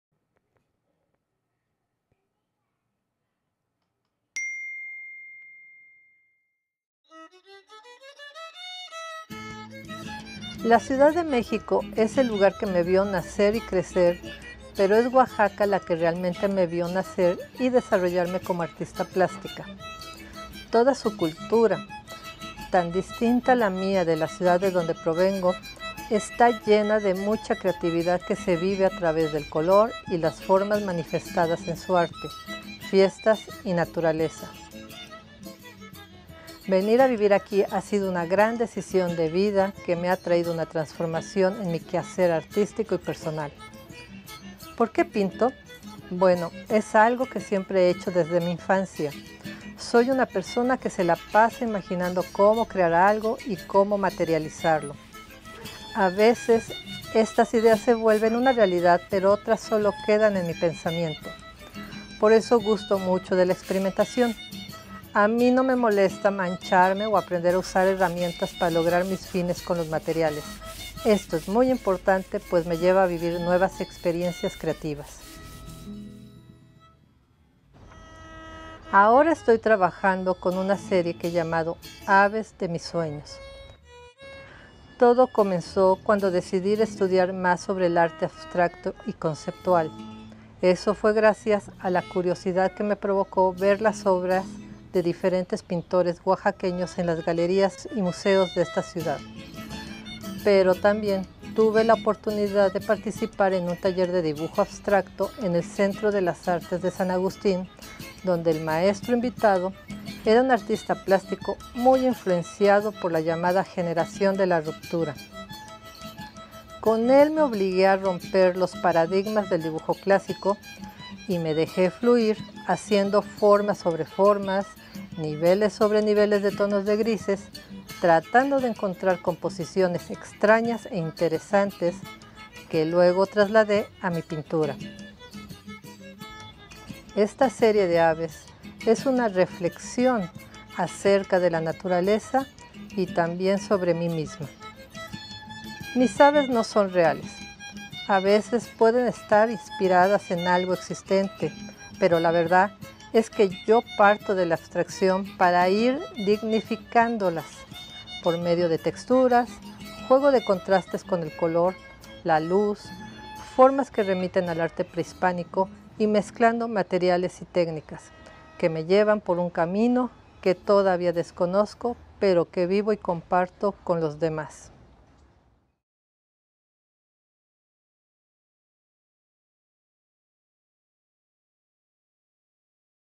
Rocío Olguín Nava's Artist's Statement
This video is about the influences and process of Rocío Olguín Nava, a Oaxaca-based visual artist.